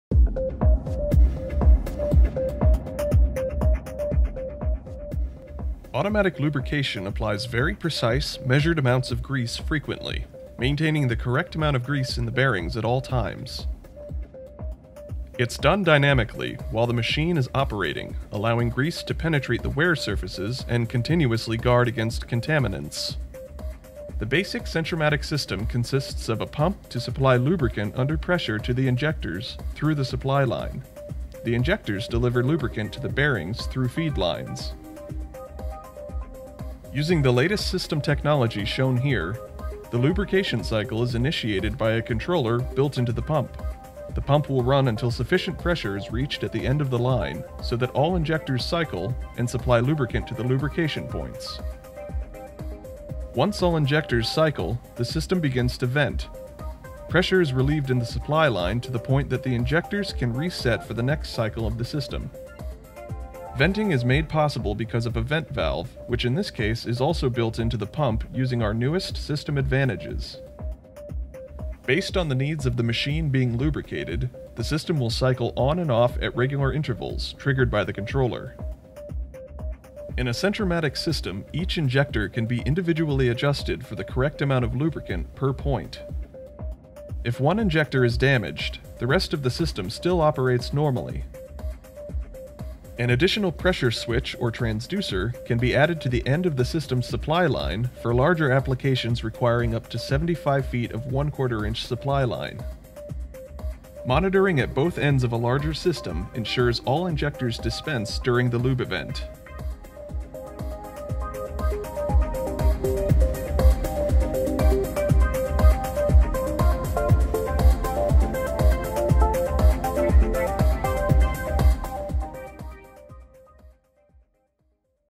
CentroMatic Bearing
Lincoln Industrial's CentroMatic system: single-line automatic lubrication in a bearing.